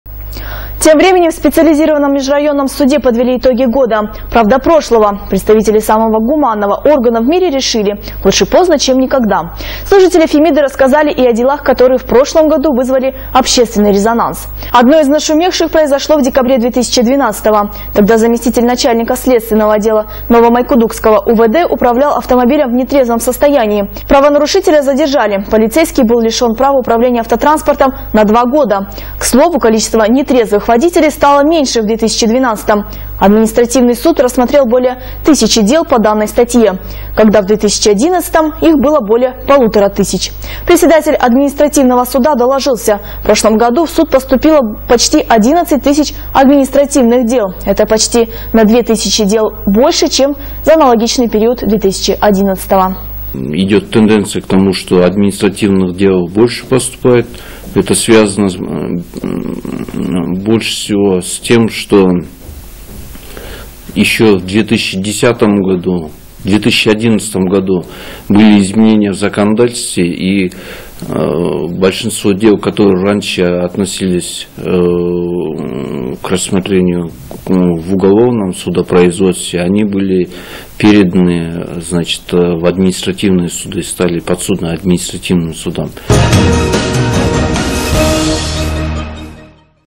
Суды. Итоги года 16.01.2013
Служители фемиды рассказали и о делах, которые в прошлом году вызвали общественный резонанс. Одно из нашумевших произошло в декабре 2012. Тогда заместитель начальника следственного отдела новомайкудукского УВД управлял автомобилем в нетрезвом состоянии.